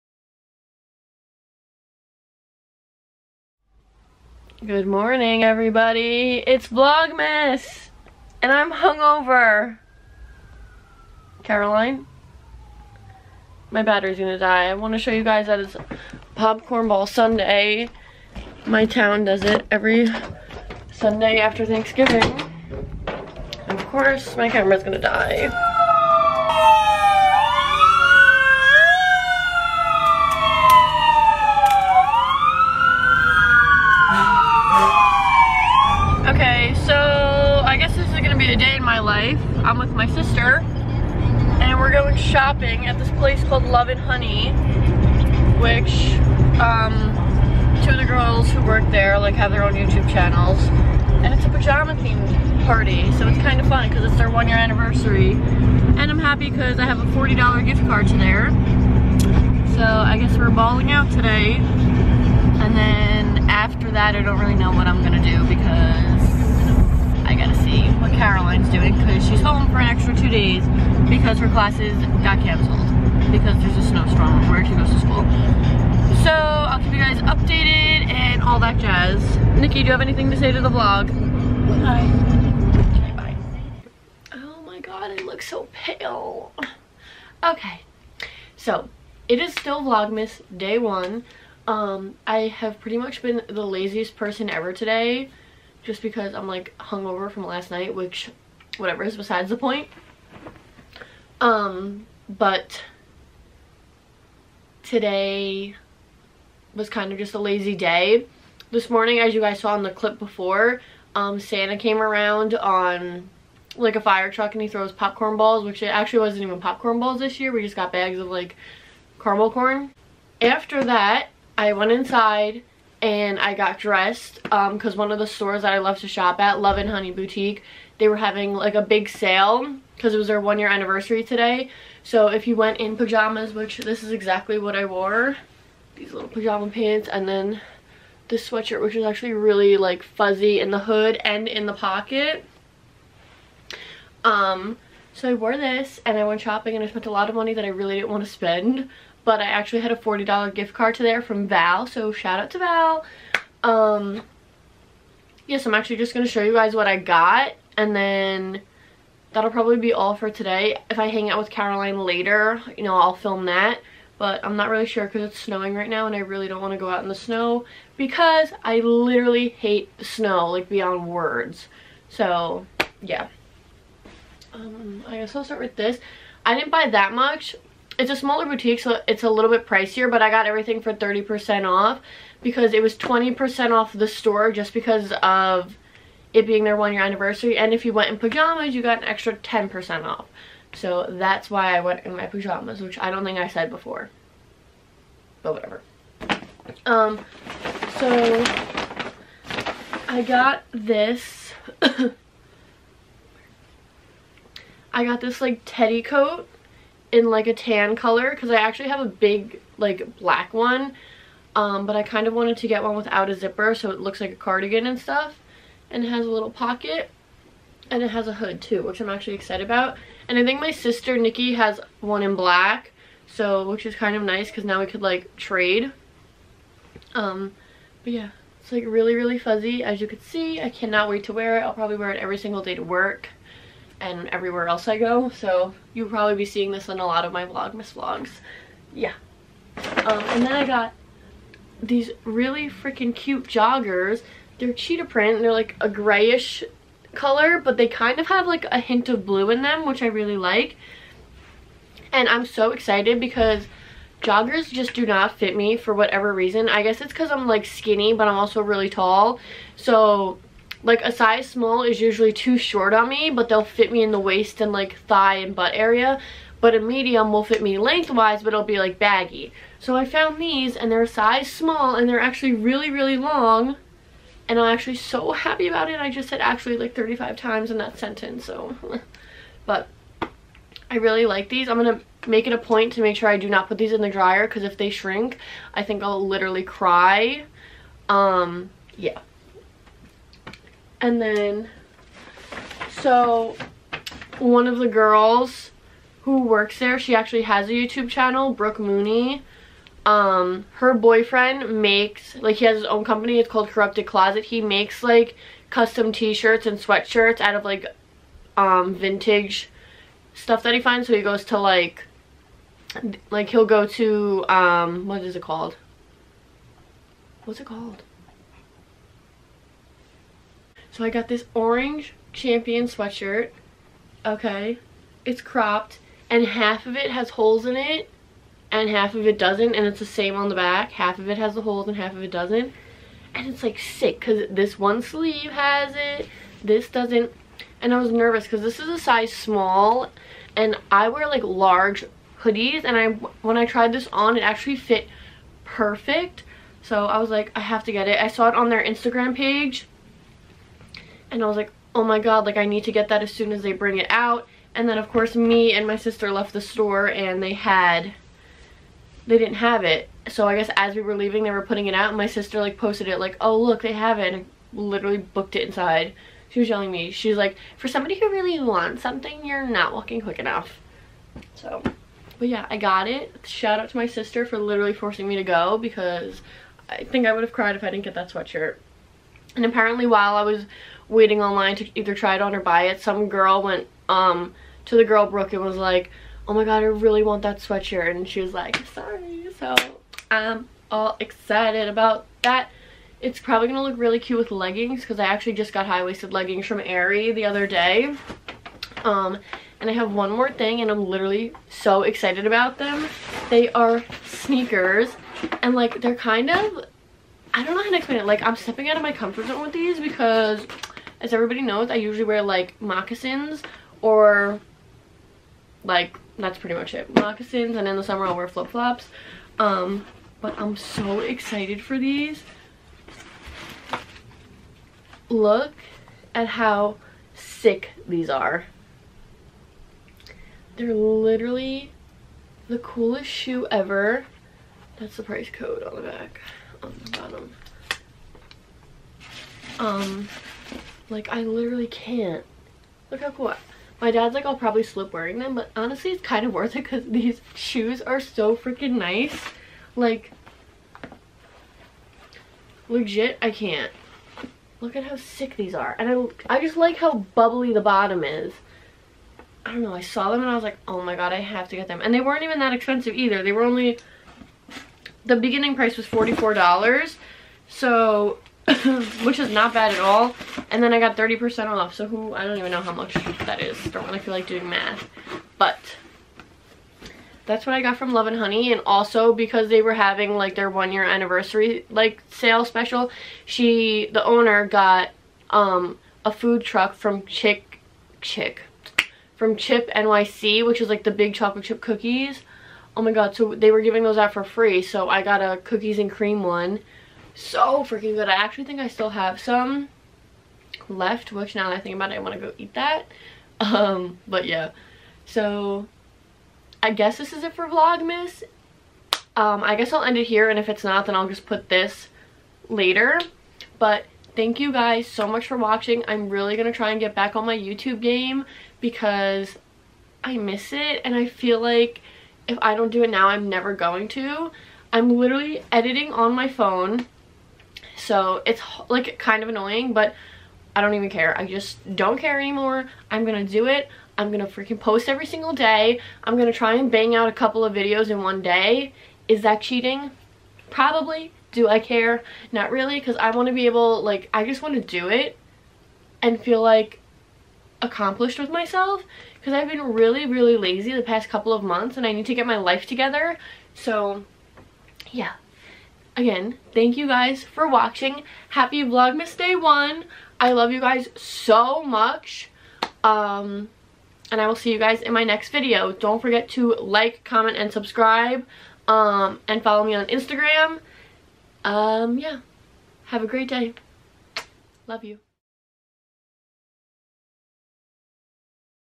VLOGMAS DAY 1 | hungover and mini shopping haul
hey guys! happy vlogmas ❤️💚!!! im so glad to be back and can't wait to be posting everyday!

love u all xoxo
-marina